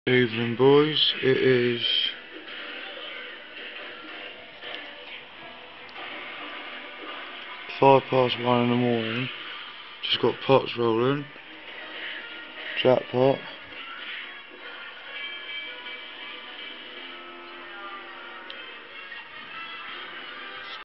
Sky Vegas Late Night win
only put in £22 got a big roll in was going to collect 200 then these bad boys rolled in

Fuck u haters lol

Jackpottyforums.co.uk